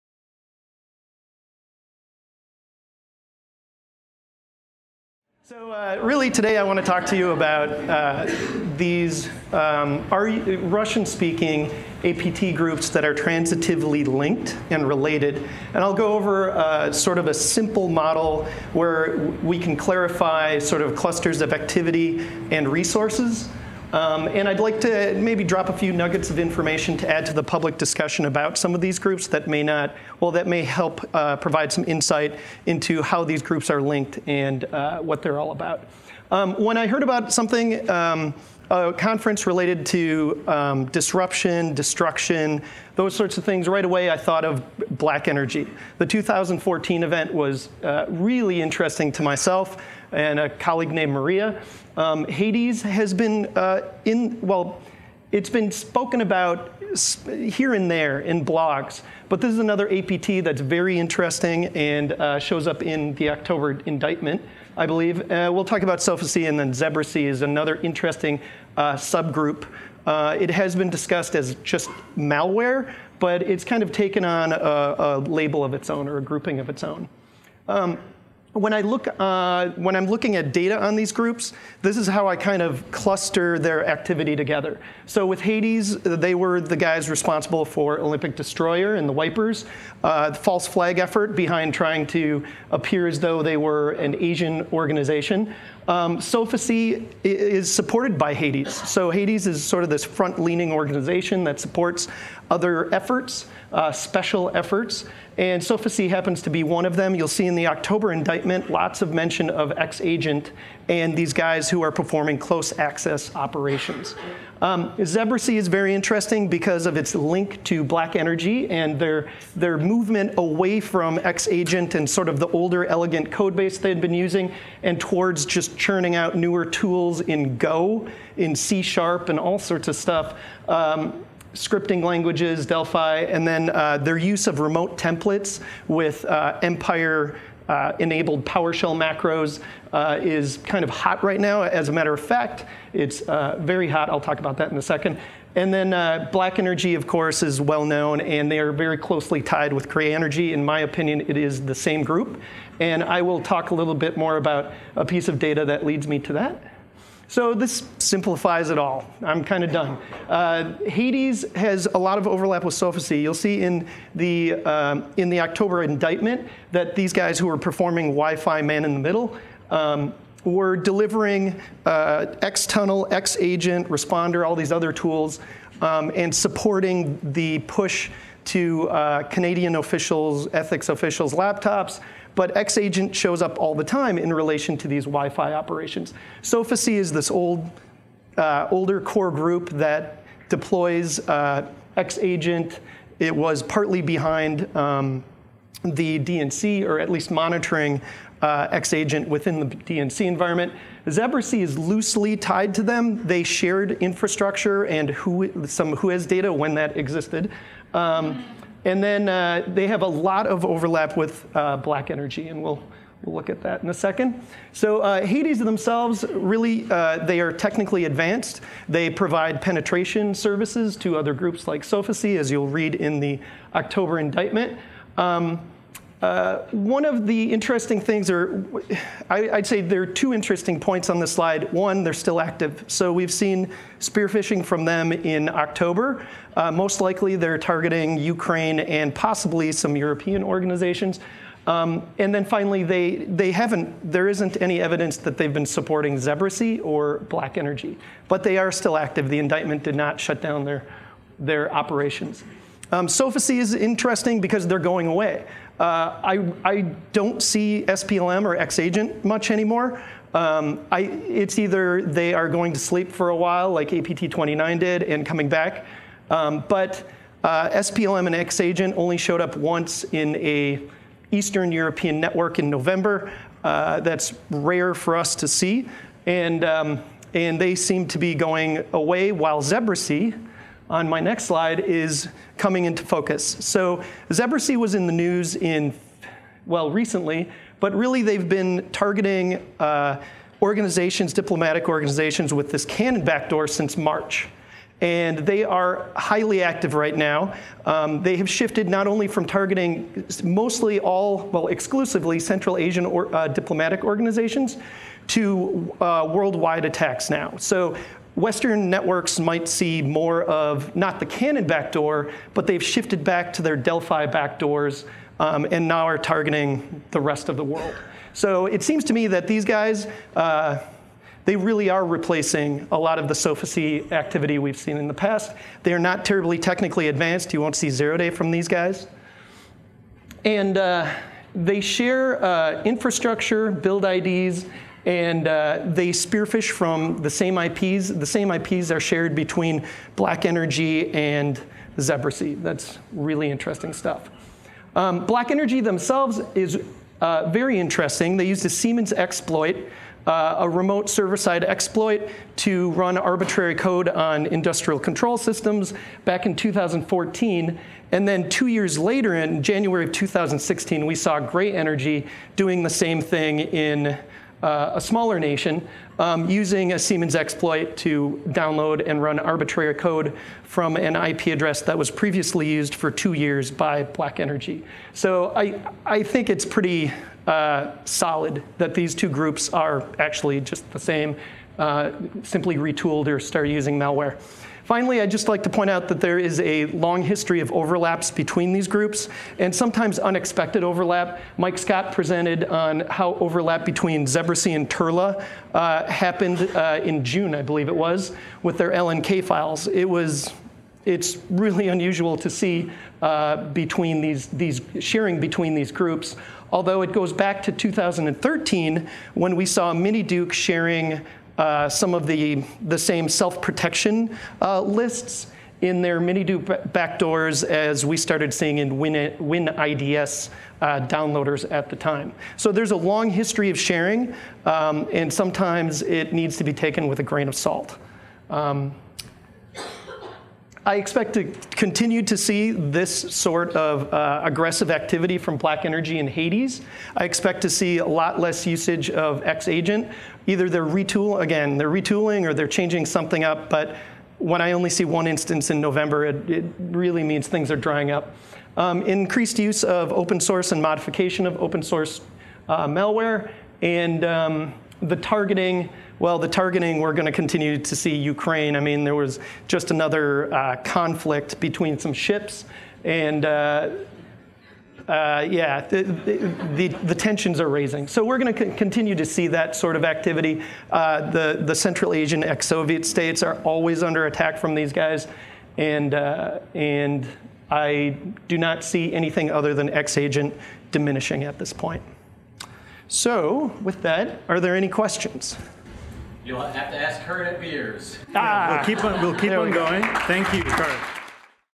Barely Whispering
CYBERWARCON, November 28, 2018, Arlington, VA

Presentation by Kurt Baumgartner, Kaspersky

Data points over the past couple of years provide quiet hints of links between what was previously considered Sofacy, Zebrocy, and Black Energy. Now, evidence seems to suggest yet another clustering. Some of these connectors breezed through public discussion and some have not. However, when data unexpectedly overlaps between even Turla and Zebrocy, these weak connections, evaluations of motivations and targeting, and clustering itself, need to be carefully considered. Another look at the forest in the context of publicly available data, along with newer targeting and malware data, may still aid in seeing the forest while looking at the trees.